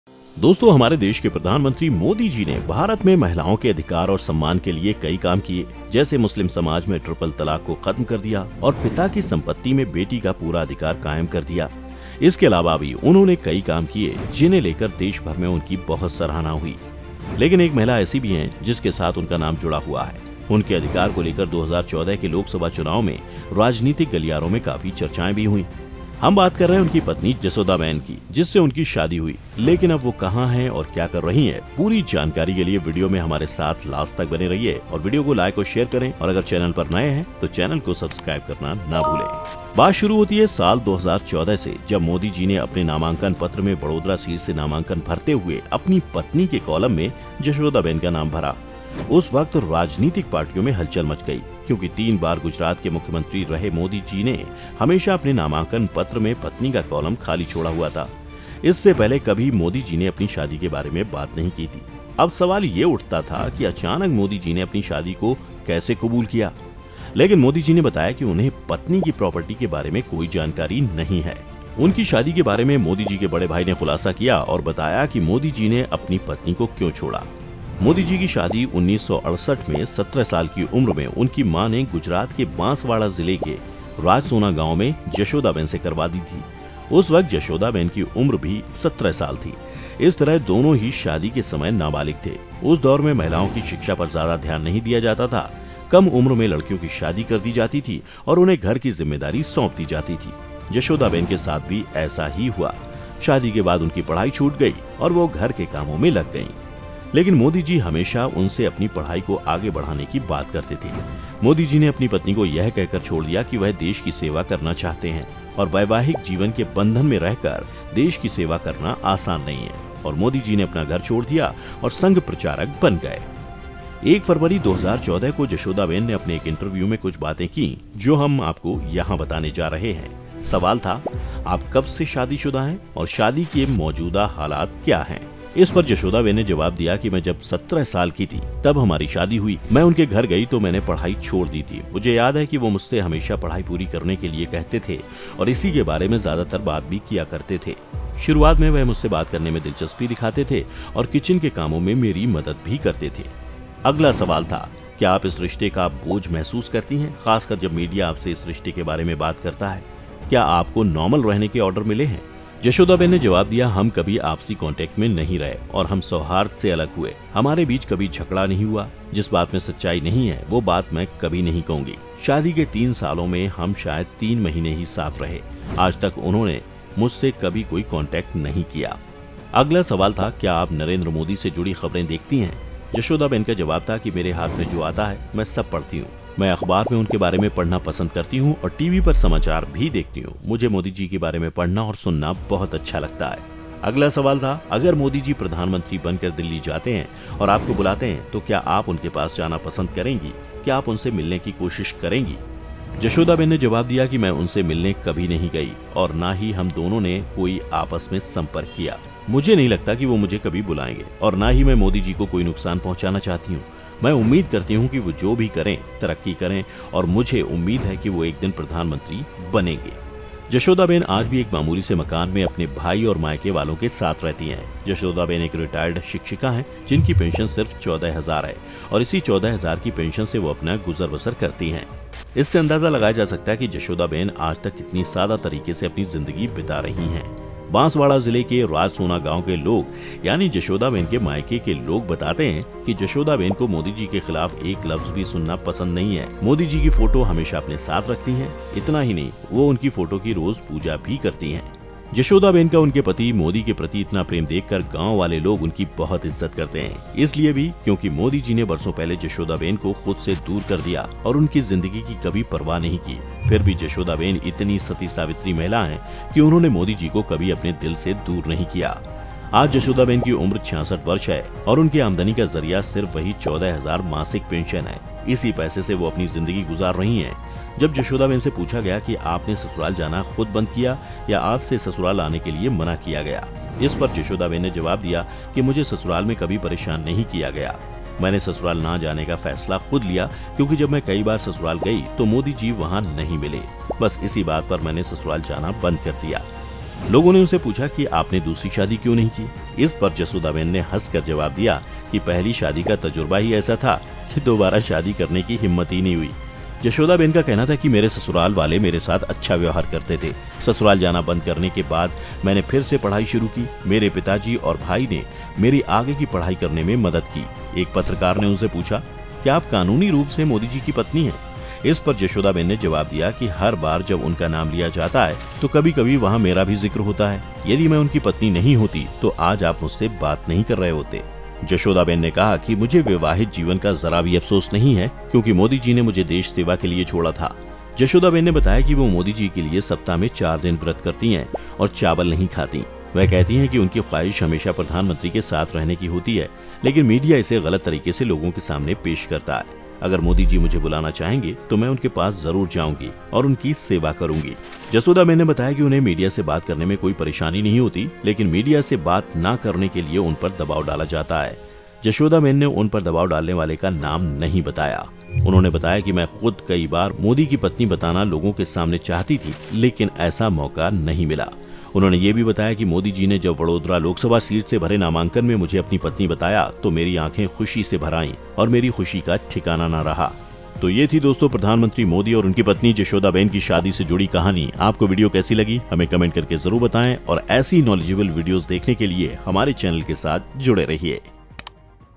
आखिर क्यों PM Modi ने 50 साल तक छुपाई अपनी शादी? | Truth about PM Modi's Marriage with Jashodaben
Video Title : आखिर क्यों PM Modi ने 50 साल तक छुपाई अपनी शादी? | Truth about PM Modi's Marriage with Jashodaben
Language  : Hindi